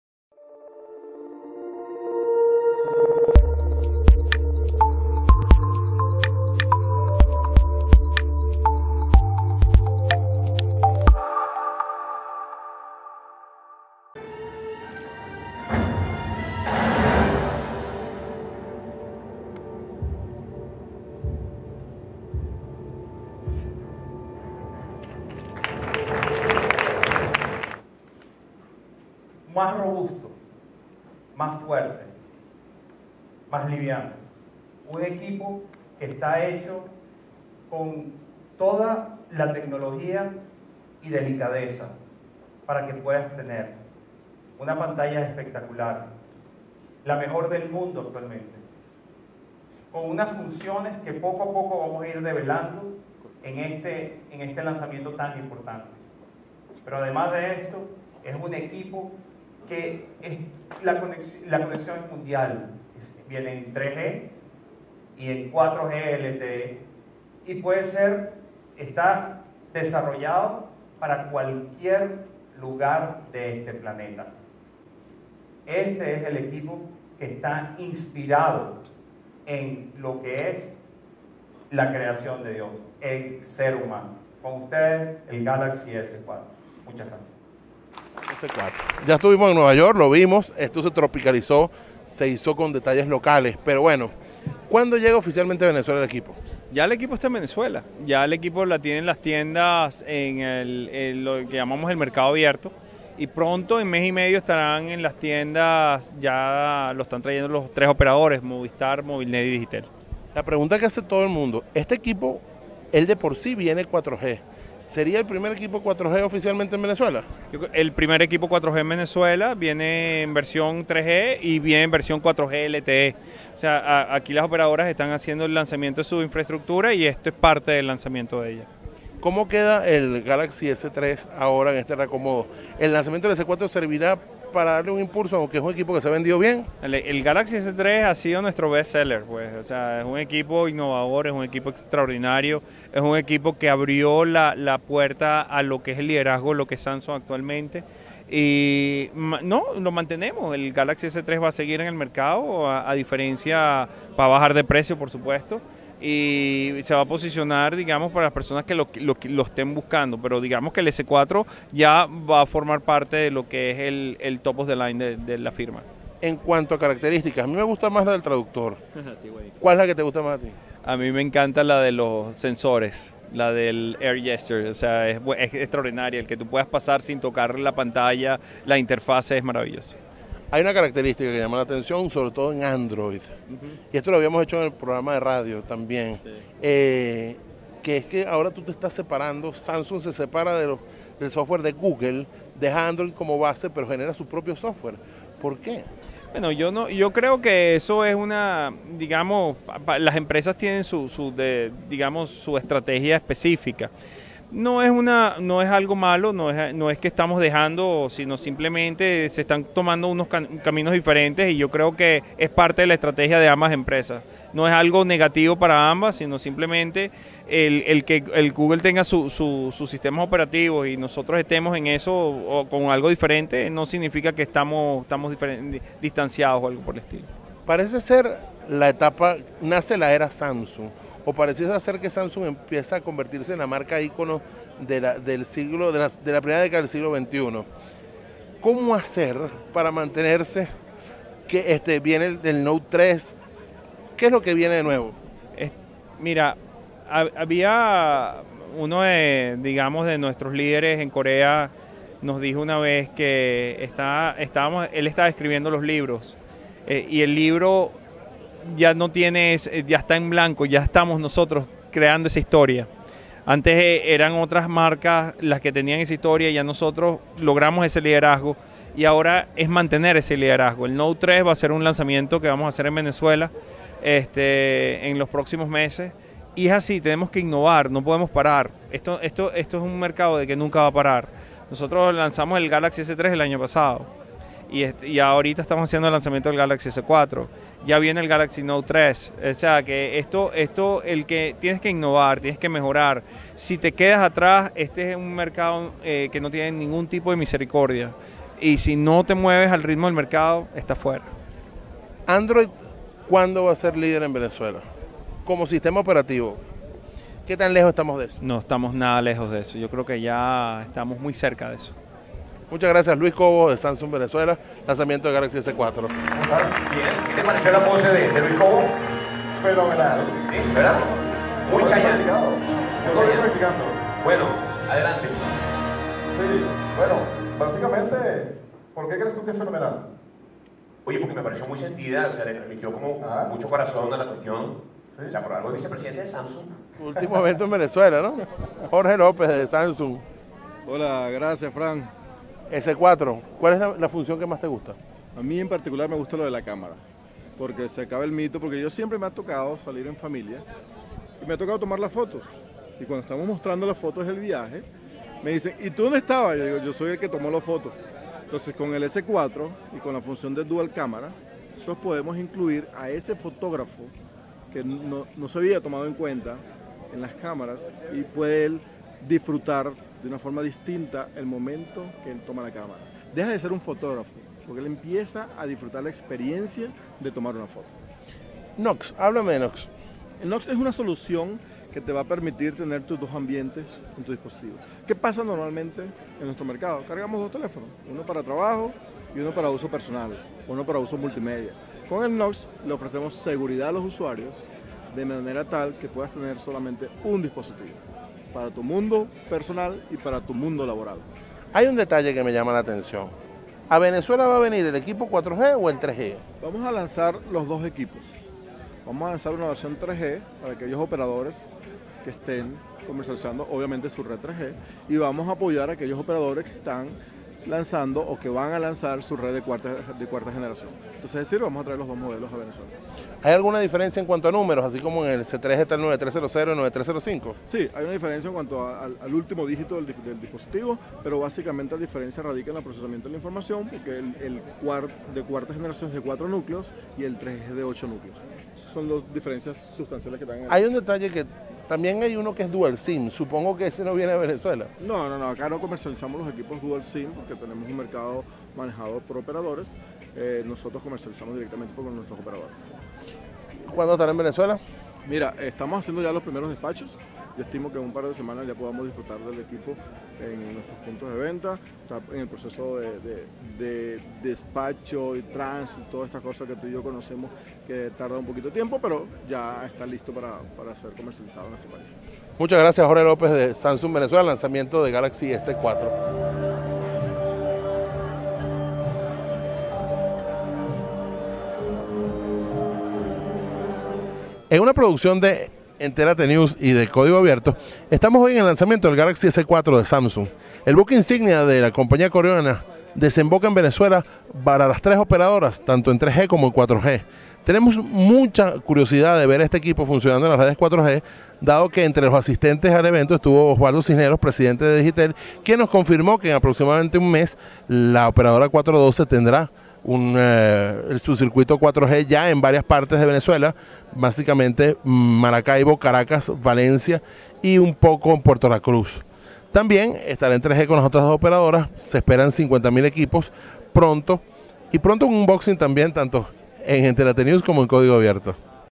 TBT: Lanzamiento del Samsung Galaxy S4 en Venezuela
Este fue la reseña del lanzamiento del Galaxy S4 que hicieramos en 2013 junto a Tecnologizados.Com. Entrevistas a Luis Cobo y Jorge López en esa época de Samsung Venezuela.